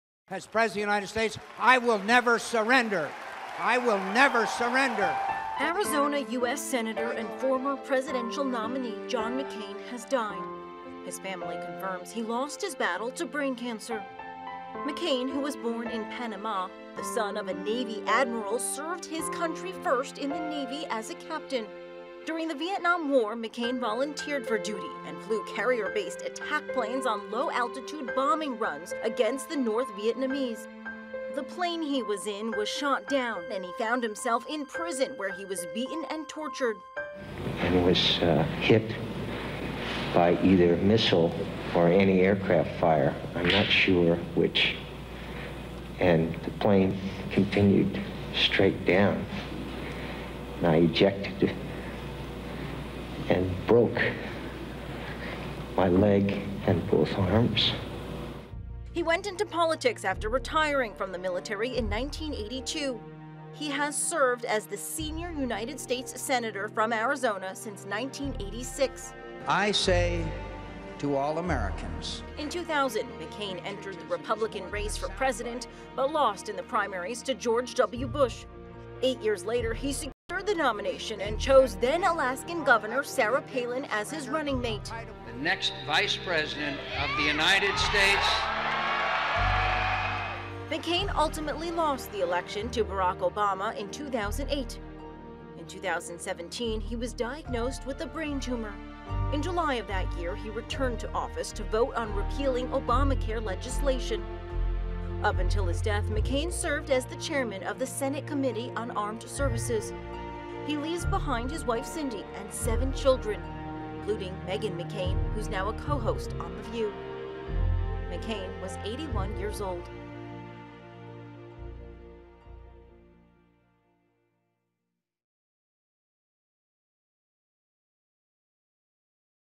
Arizona Sen. John McCain Dies of Brain Cancer at 81
Arizona Senator and former presidential nominee John McCain has died. He was a Navy pilot in 1967 when his plane was shot down in Vietnam. He spent five years as a prisoner of war. McCain was elected to the Senate in 1986. In 2000, he entered the Republican race for president but lost in the primaries to George W. Bush. In 2008, he became the Republican nominee for president, but lost the general election to Barack Obama. In 2017, he was diagnosed with brain cancer. John McCain was 81.